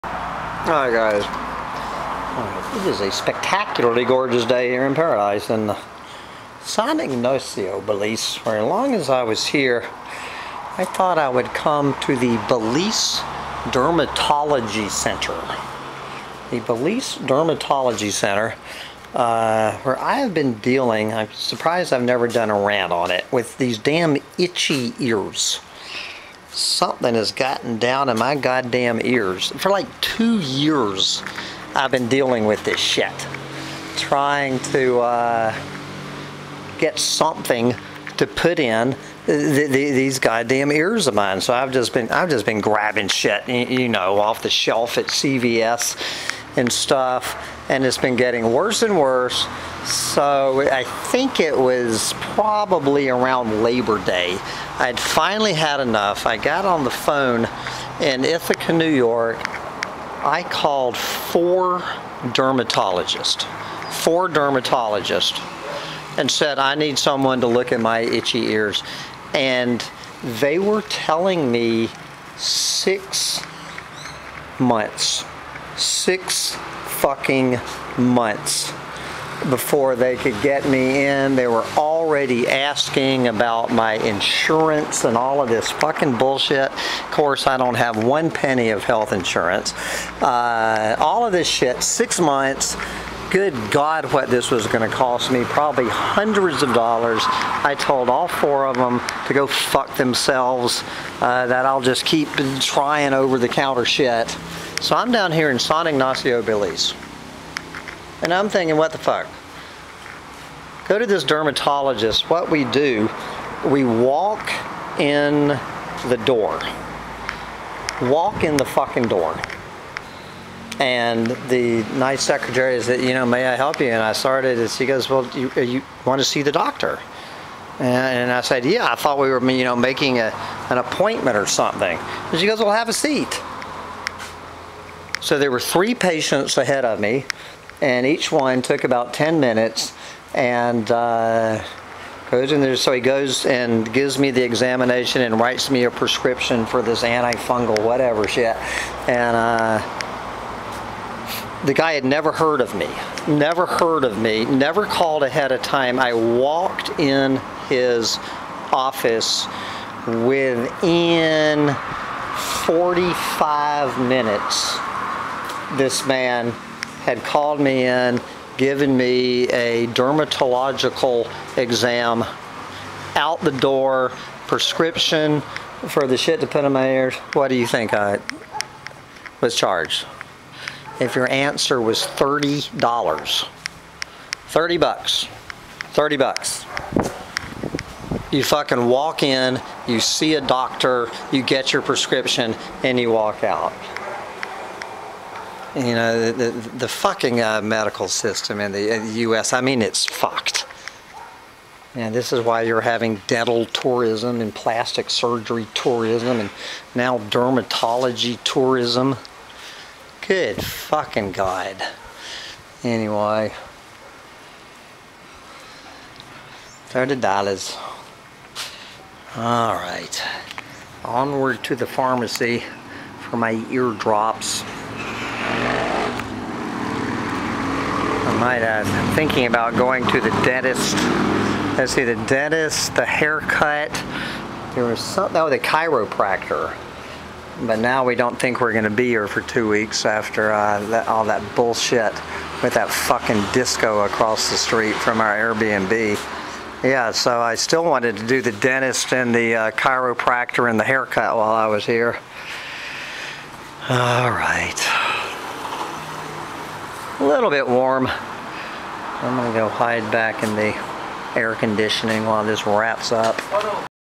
Hambone in Belize, Ch. 13: A Clueless American Visits a Doctor in Another Country
I know there is a reason you can do this in Belize, but not in the U.S., I just can't remember what it is.